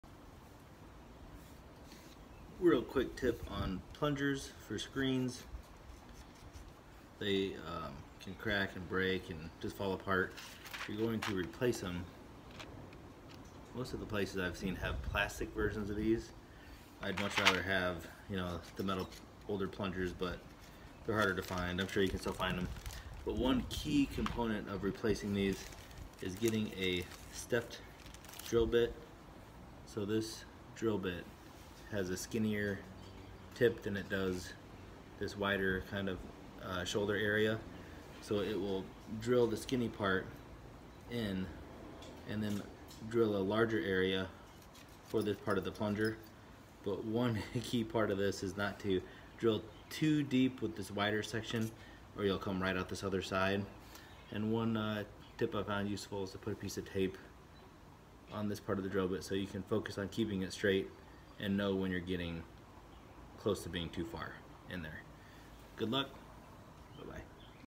Window Screen Plunger Install
When installing a window screen plunger use a stepped drill bit. Using a piece of tape on the drillbit will help you know how deep you have drilled.